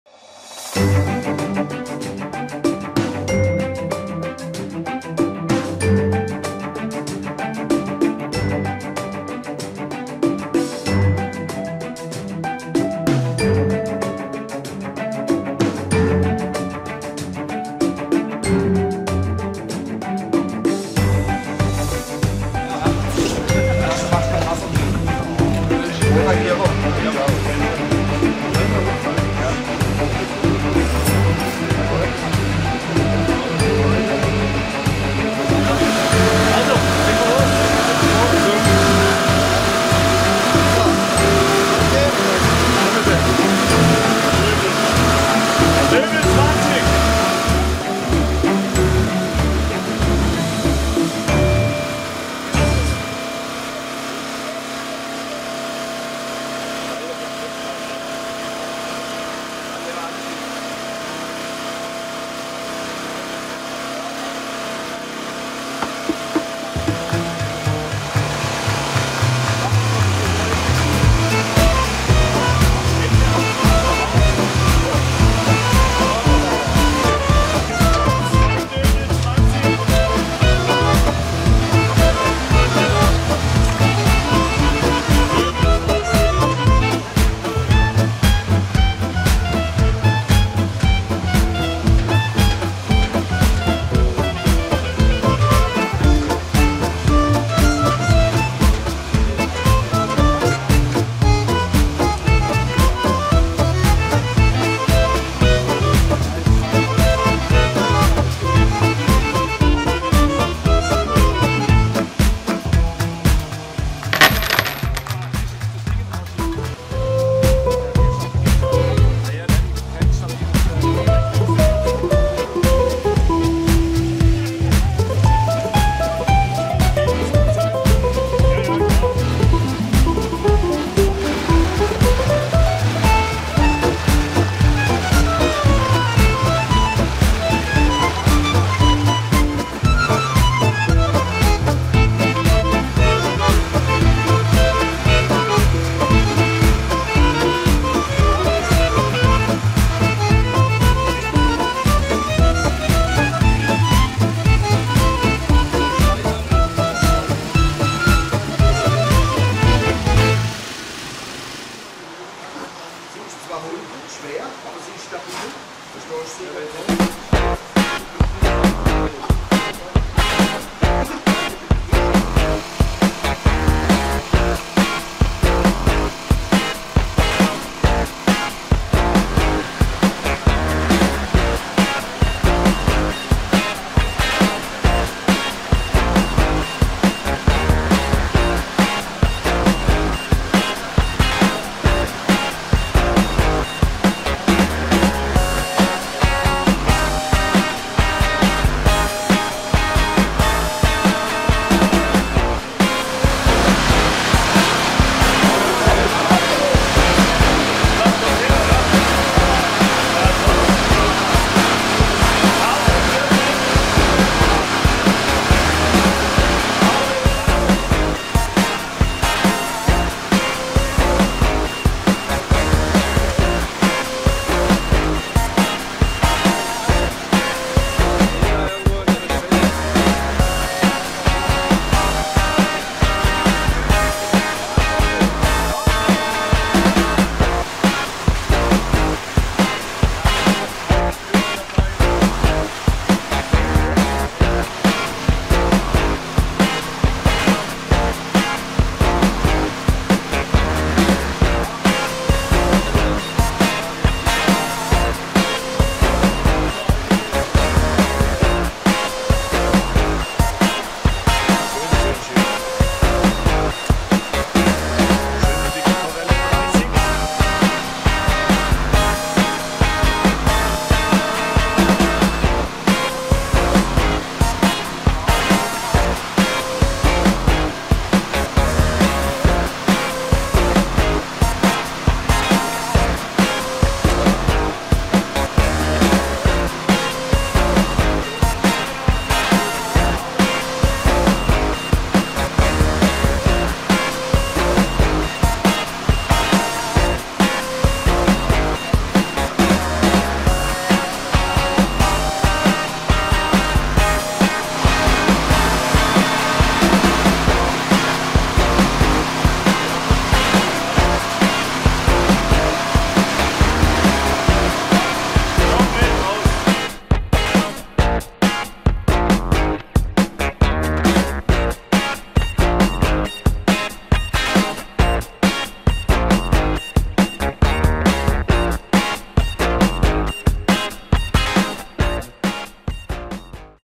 Elektrofischen Kraftwerkskanal Emmendingen 2014
Nach dem teilweisen Ablassen des Mühlkanals in Emmendingen wurde eine Bestandserfassung der vorhandenen Fische durch Elektrofischen durchgeführt. Der Landesfischereiverband Baden und sein Mitglied, der Anglerverein Emmendingen haben diese Aktion gemeinsam erfolgreich gemacht.